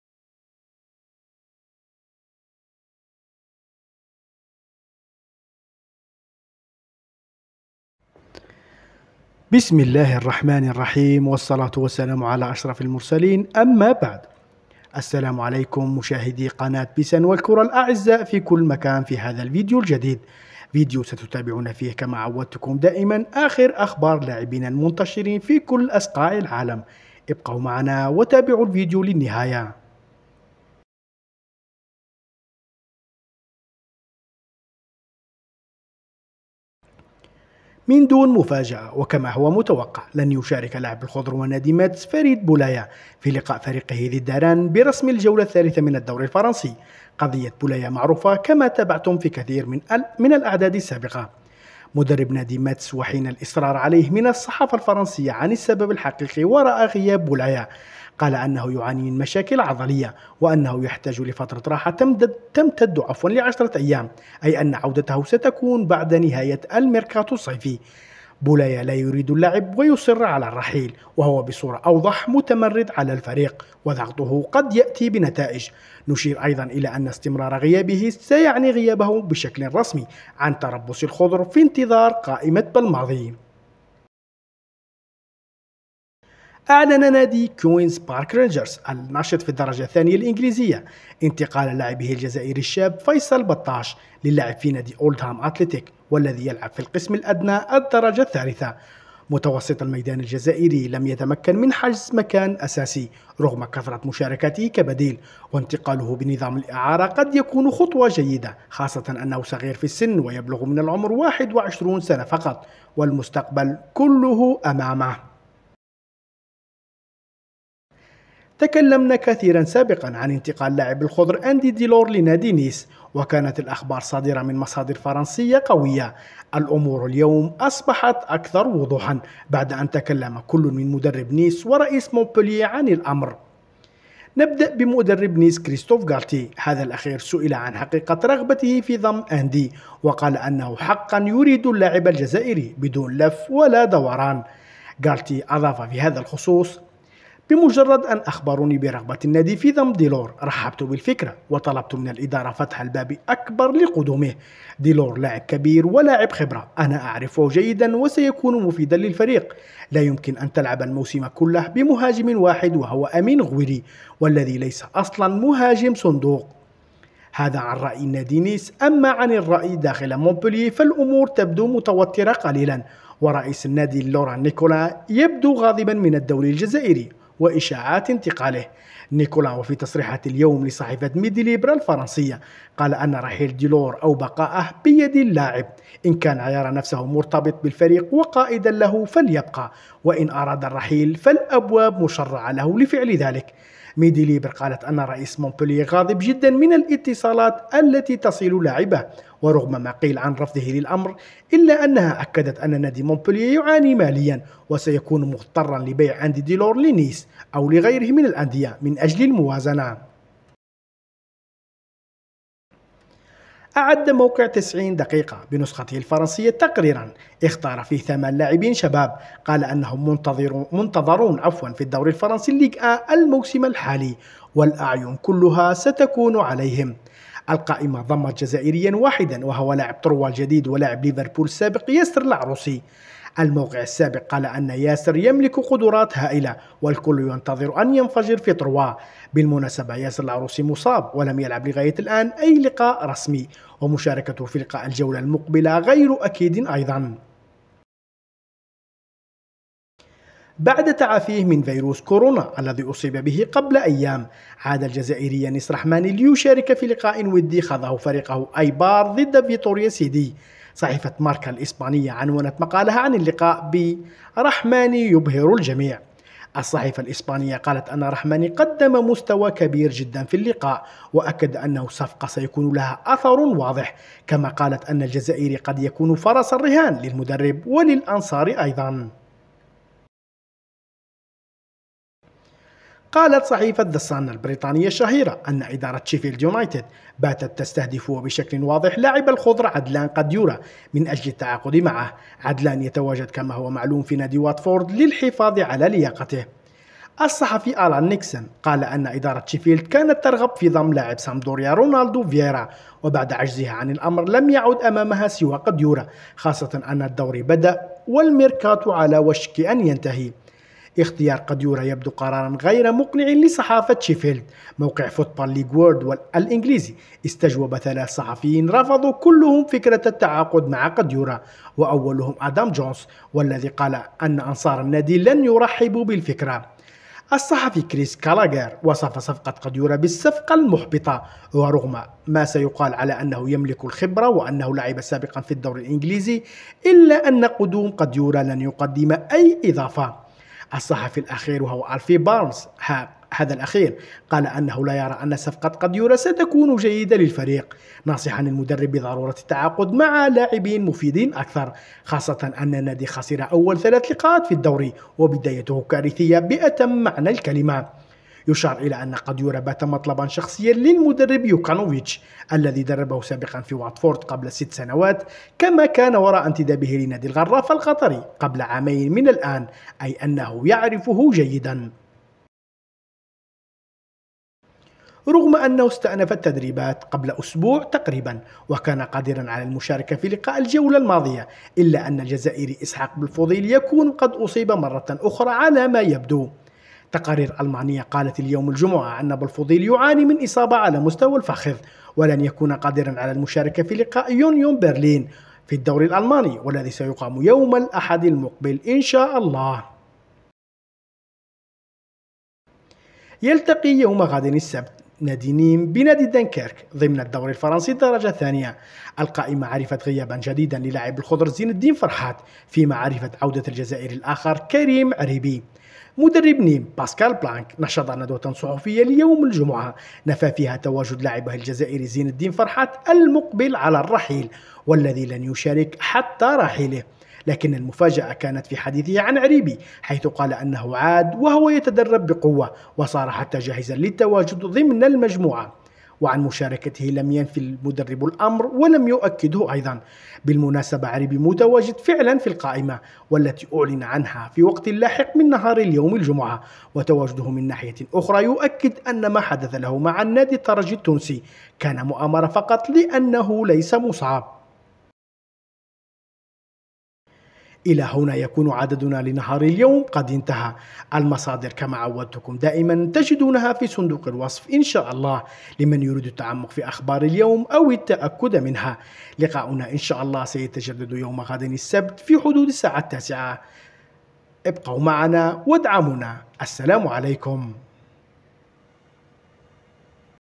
ديلور يقترب من الرحيل مدرب نيس يوضح كل شيء/ قديورة بولاية بطاش لعروسي بلفوضيل رحماني عريبي فرحات
المصادر